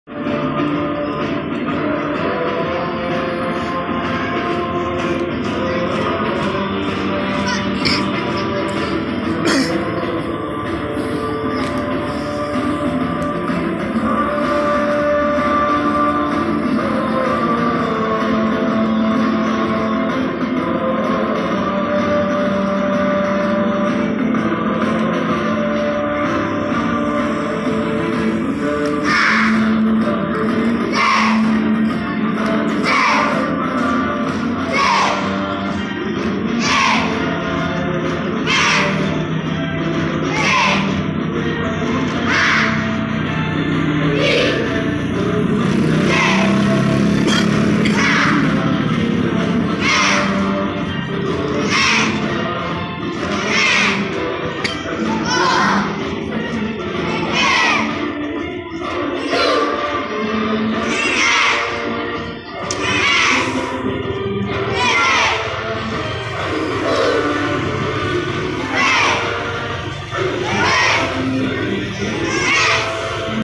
Semaphore pormasi tulisan Allah di SD IT AL MANAR Takengon Aceh Tengah
Dalam rangka penyambutan tim asesor akreditasi sekolah, siswa siswi sd it al manar takengon menampilkan formasi tarian semaphore berbentuk allah. Simak videonya.